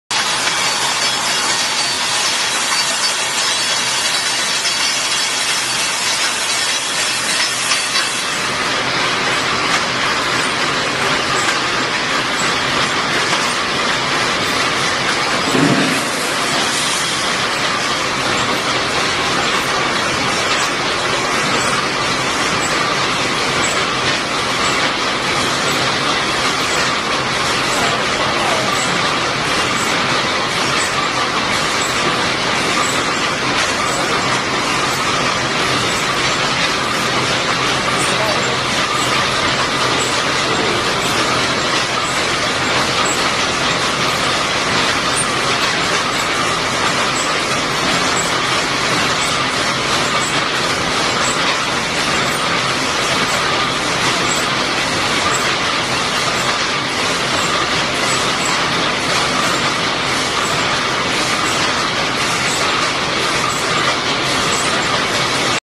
Automatic chicken cage welded wire mesh machine from Lina,CHAOYI HTK
Automatic chicken cage welded wire mesh machine from Lina,CHAOYI-HTK. High Speed, High quality.